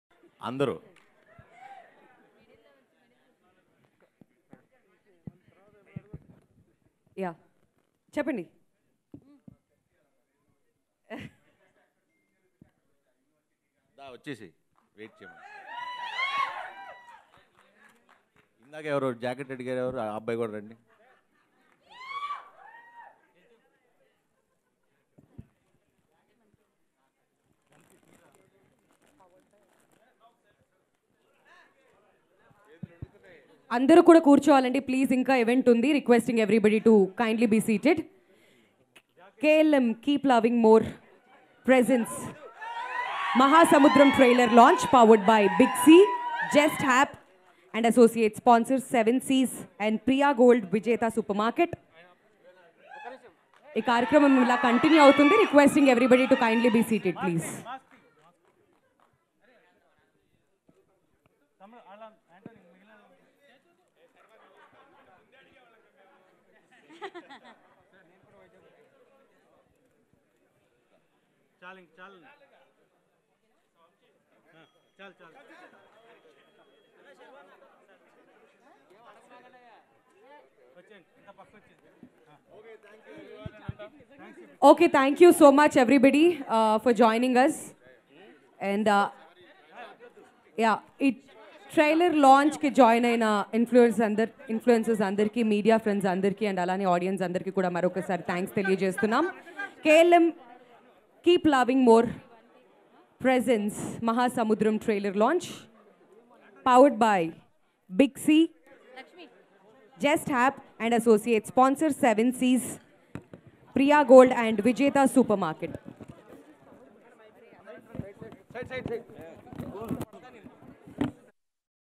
Sharwanand Gave His Jacket To a Fan At Maha Samudram Trailer Launch | Andhra Life Tv
Watch Here : Sharwanand Gave His Jacket To a Fan At Maha Samudram Trailer Launch